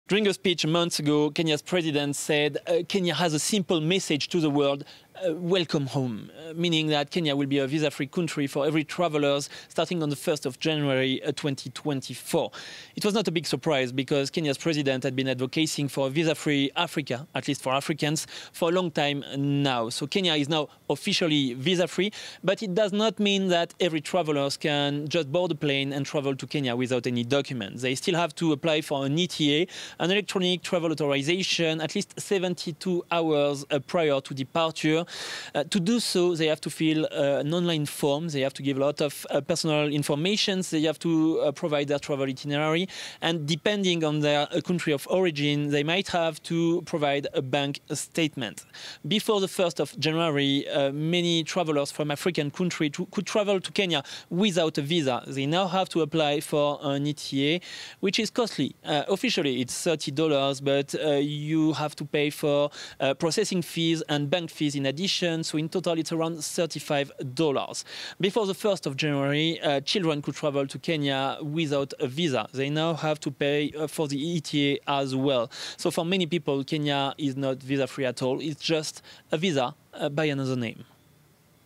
Kenya says first 'visa-free' tourists arrive in country • FRANCE 24 English
Kenya said Friday it had welcomed the first batch of foreign tourists who arrived under a simplified entry system it hopes will encourage more visitors. FRANCE 24's Bastien Renouil explains.